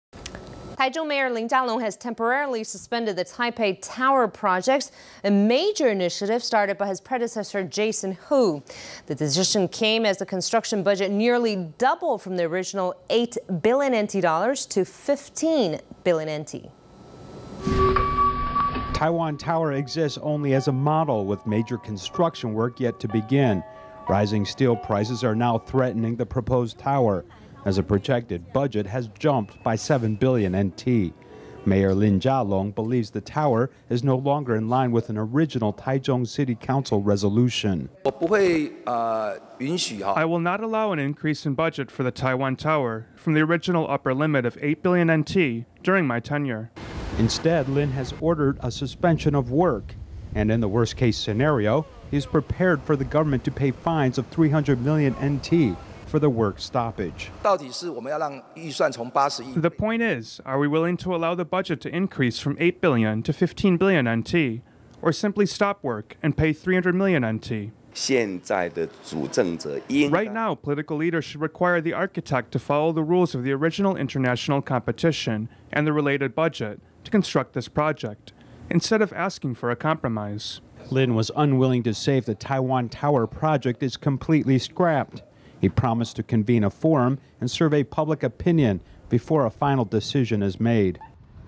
Taichung mayor halts Taiwan Tower project over budget concerns
Taichung Mayor Lin Chia-lung has temporarily suspended the Taipei Tower project, a major initiative started by his predecessor Jason Hu. The decision came as the construction budget nearly doubled from the original NT$8 billion to NT$15 billion. Taiwan Tower exists only as a model with major construction work yet to begin. Rising steel prices are now threatening the proposed tower, as the projected budget has jumped by NT$7 billion. Mayor Lin Chia-lung believes the tower is no longer in line with an original Taichung City Council resolution.Lin Chia-lungTaichung MayorI will not allow an increase in budget for the Taiwan Tower from the original upper limit of NT$8 billion during my tenure.Instead, Lin has ordered a suspension of work. And, in the worst-case scenario, he is prepared for the government to pay fines of NT$300 million for the work stoppage.Lin Chia-lungTaichung MayorThe point is, are we willing to allow the budget to increase from NT$8 billion to NT$15 billion, or simply stop work and pay NT$300 million?Hsiao Chia-chiFormer Taichung Deputy MayorRight now, political leaders should require the architect to follow the rules of the original international competition and the related budget to construct this project instead of asking for a compromise.Lin was unwilling to say if the Taiwan Tower project is completely scrapped. He promised to convene a forum and survey public opinion before a final decision is made.